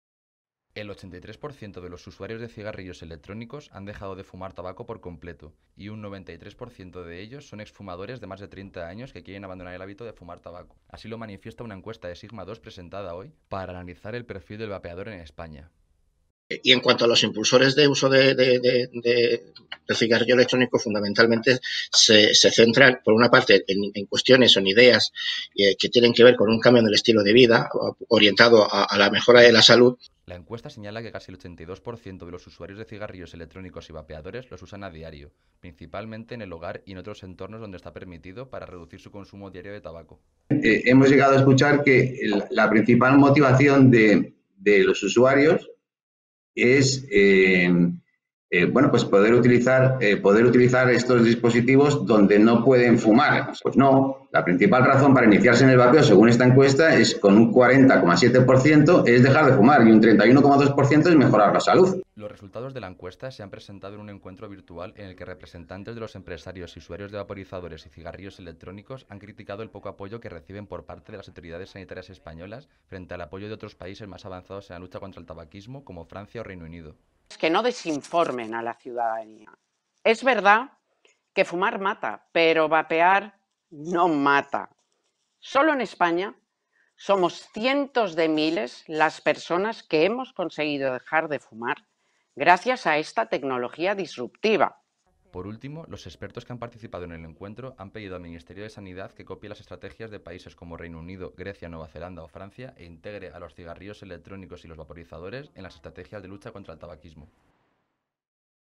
El 83% de los usuarios de cigarrillos electrónicos han dejado de fumar por completo
El 83% de los usuarios de cigarrillos electrónicos han dejado de fumar tabaco por completo. y un 93% de ellos son exfumadores de más de 30 años que quieren abandonar el hábito de fumar tabaco. Así lo manifiesta una encuesta de Sigmados presentada hoy para analizar el perfil del vapeador en España.
La encuesta señala que casi el 82% de los usuarios de cigarrillos electrónicos y vapeadores los usan a diario, principalmente en el hogar y en otros entornos donde está permitido para reducir su consumo diario de tabaco.
Los resultados de la encuesta se han presentado en un encuentro virtual en el que representantes de los empresarios y usuarios de vaporizadores y cigarrillos electrónicos han criticado el poco apoyo que reciben por parte de las autoridades sanitarias españolas frente al apoyo de otros países más avanzados en la lucha contra el tabaquismo como Francia o Reino Unido.
Por último, los expertos que han participado en el encuentro han pedido al Ministerio de Sanidad que copie las estrategias de países como Reino Unido, Grecia, Nueva Zelanda o Francia e integre a los cigarrillos electrónicos y los vaporizadores en las estrategias de lucha contra el tabaquismo.